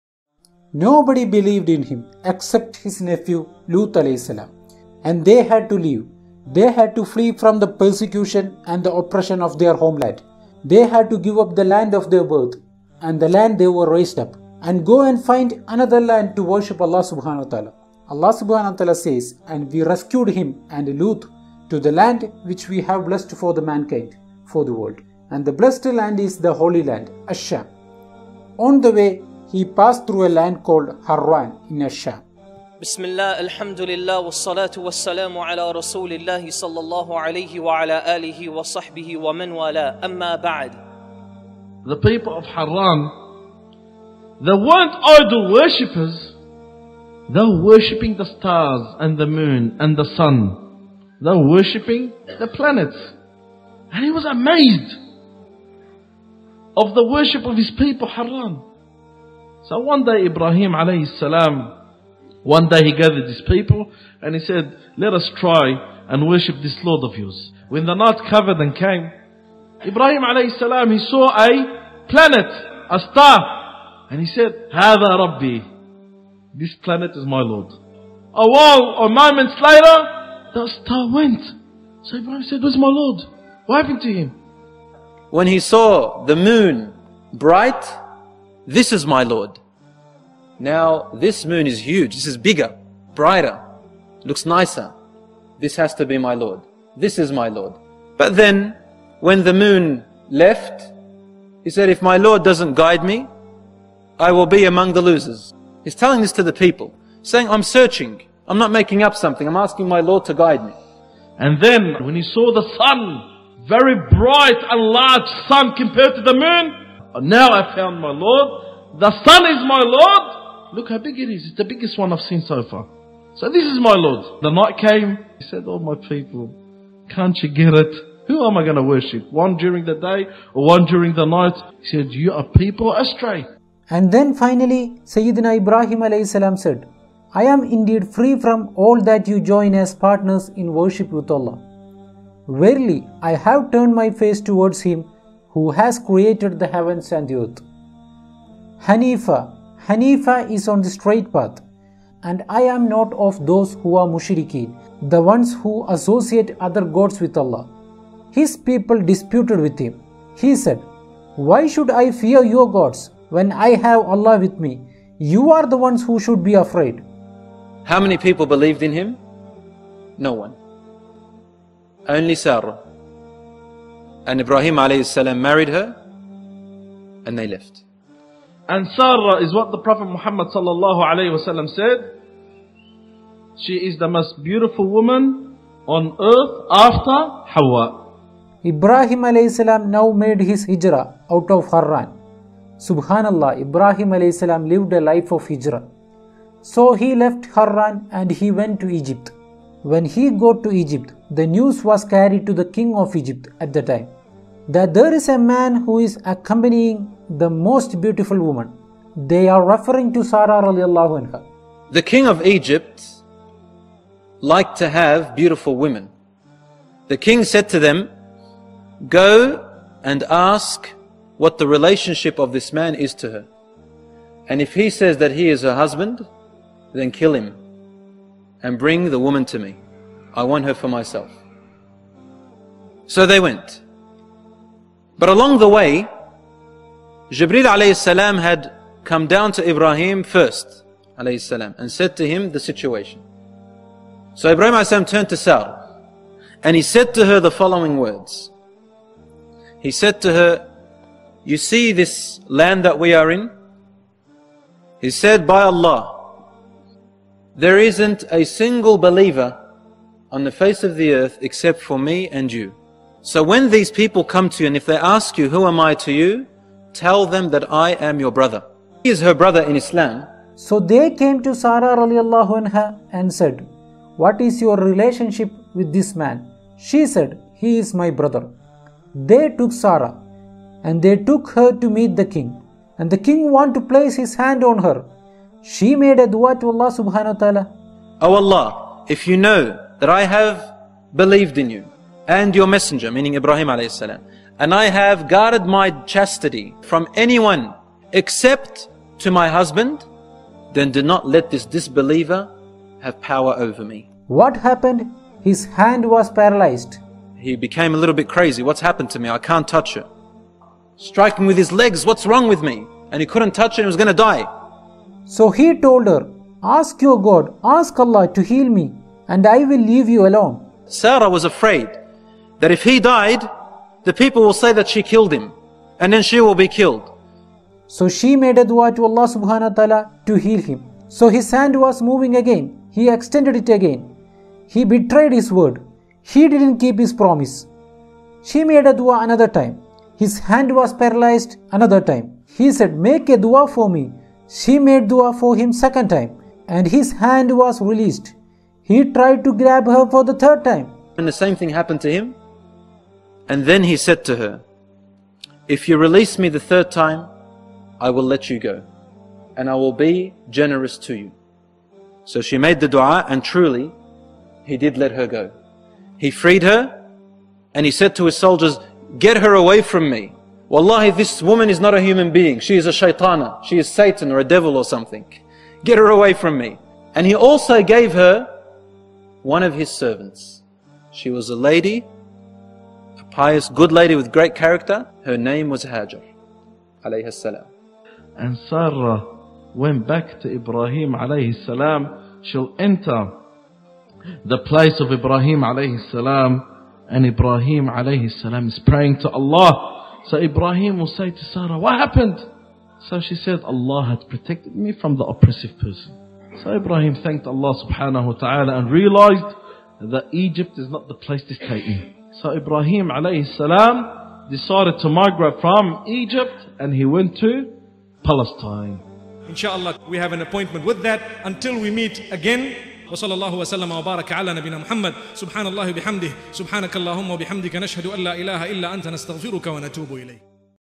The Story of Prophet Ibrahim AS (Abraham) In Harran & Egypt - Khalilullah (Friend of Allah) Part 3
Speakers:- Sheikh Shady Al Suleiman, Mufti Menk,  Bilal Assad, Abdul Rahman Hassan, Br. Husnayn, Br.Shuaib Khan, Saad Tasleem, Sajid Umar, Sheikh Ahmed Ali, Sheikh Mumtaz Ul Haq, Dr.Yasir Qadhi & Saeed Mohamed